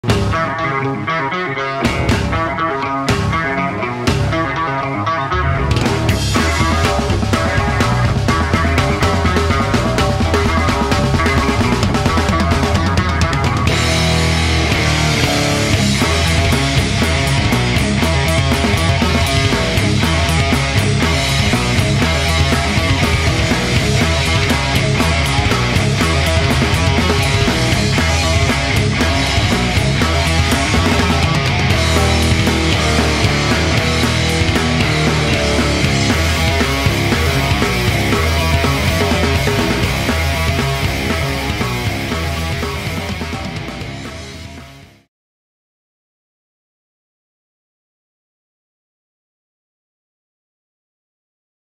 SOLARBURN
Polkadot Cadaver, Solarburn, Pipewrench, Makeshift Urn
Sunday August 27 @ Squirrel Hill Sports Bar  7 pm doors  $15